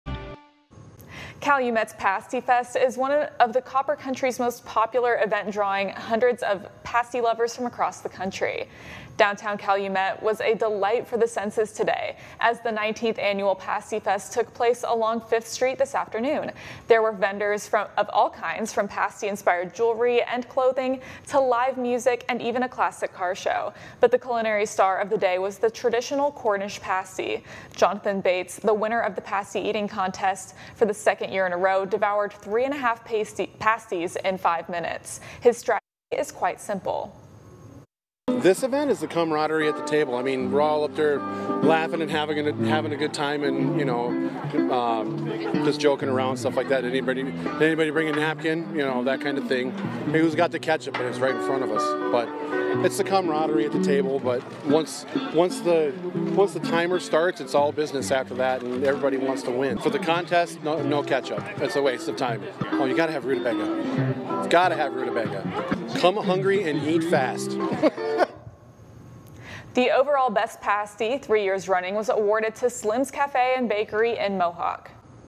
19TH annual Pasty Fest
Calumet's Pasty fest is one of the copper country's most popular events drawing hundreds of pasty lovers from across the country.